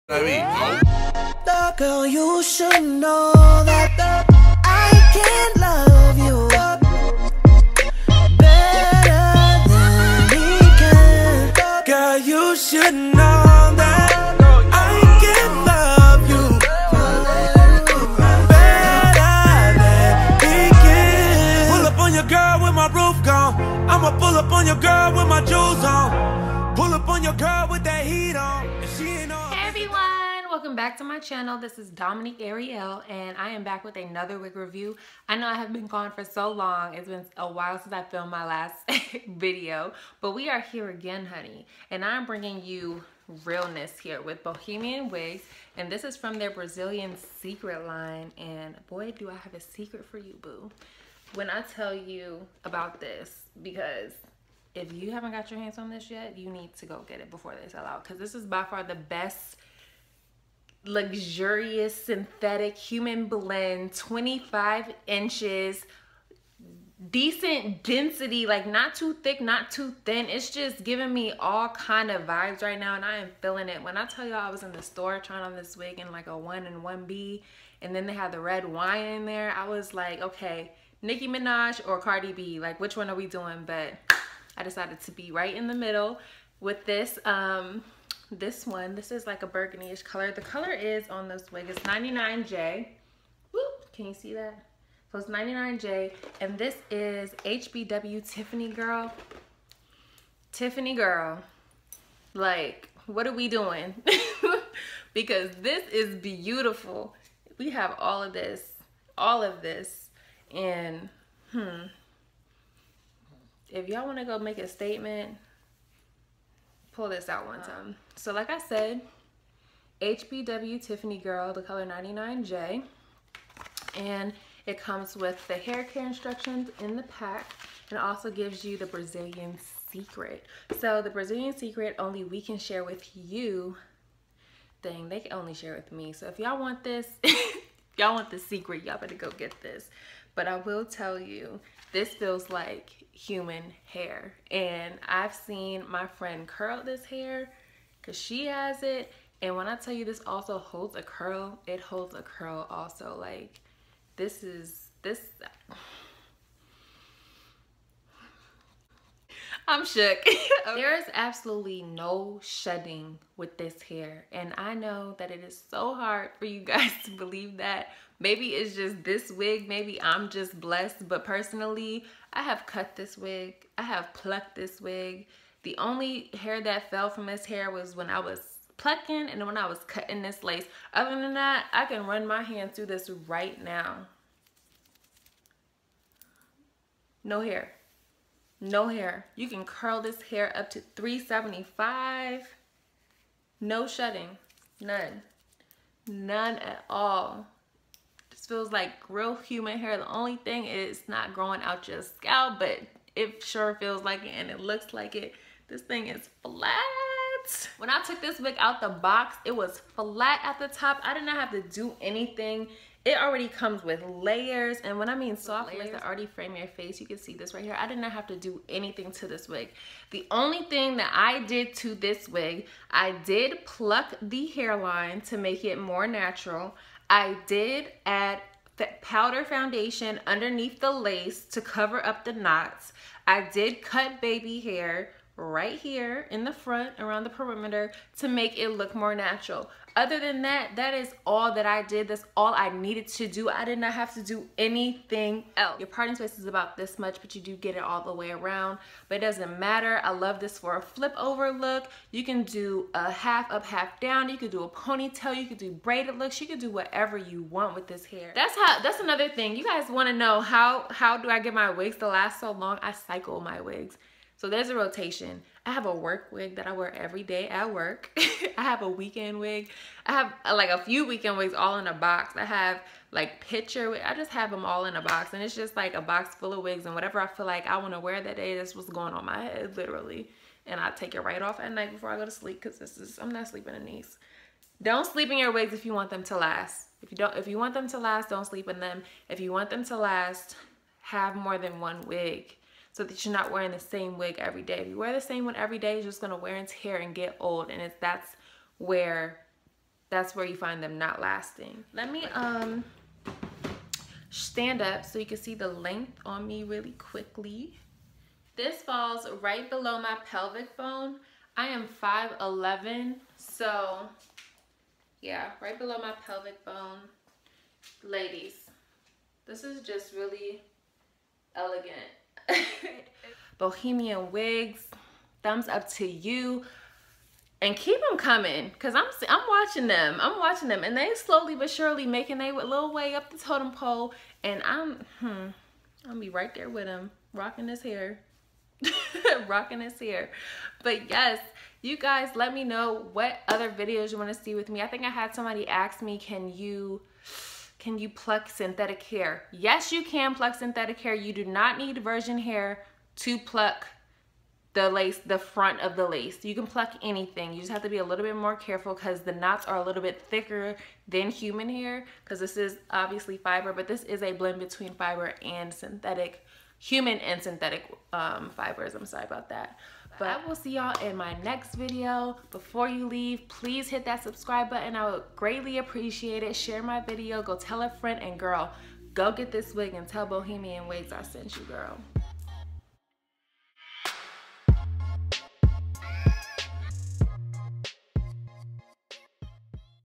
Bohemian Wig | Brazilian Secret | HBW TIFFANY GIRL
Hey gorgeous heres a flawless 24 inch moment, Talk about THE  BEST hair for fall . This unit is EVERYTHING ! Hope you enjoy .

Bohemian Wig Review . Brazilian Secret Review . HBW TIFFANY GIRL in 99J. This is a swiss lace unit with the length of  about 24inches, has three combs 2 in the front and one in the back along with an adjustable strap. A comfortable fit . I secure my unit with Gots 2b glu.
If i left out anything don't hesitate to ask in the comment section xoxo.

SC: dominiqeuarielle_